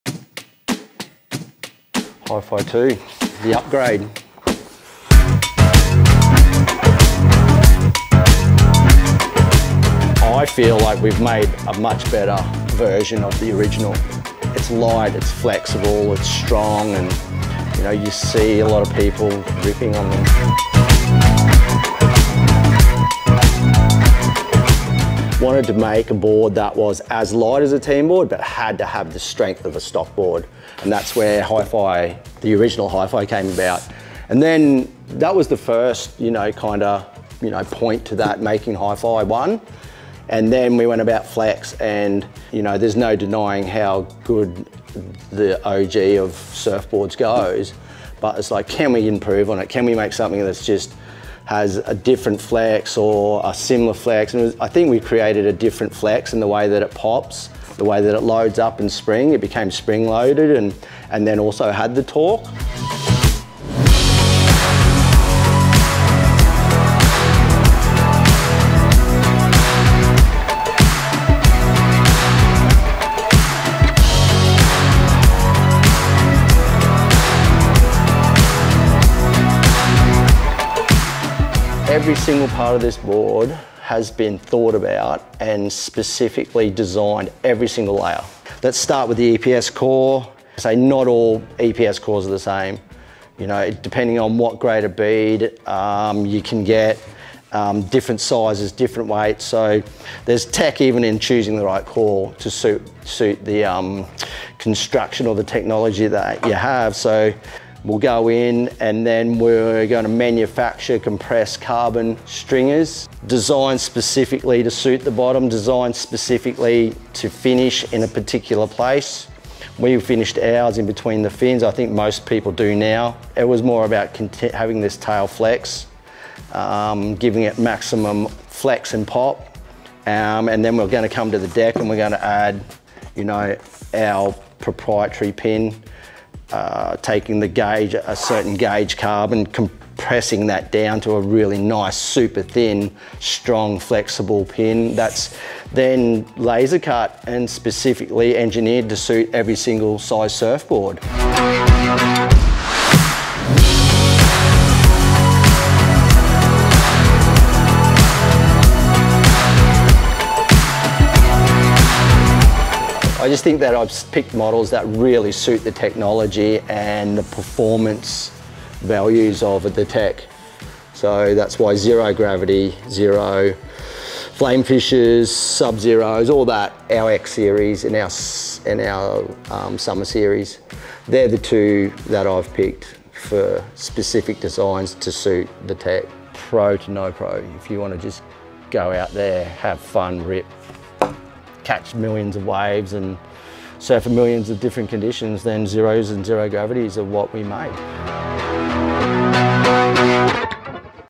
HYFI 2.0 - R&D deep dive
A closer look at the R&D behind the latest version of our HYFI construction. Having completed our original goals to be as light as a team board, but with at least the same strength as a stock board with heavier glassing, we looked further at enhancing the performance characteristics.

Every single layer of HYFI 2.0 has been specifically designed and engineered with its current purpose in mind. Starting with exactly the right type and weight of EPS core, all the way up to the scaled stringer pins made from compressed carbon. Find more details and all of the boards we make in HYFI 2.0, below: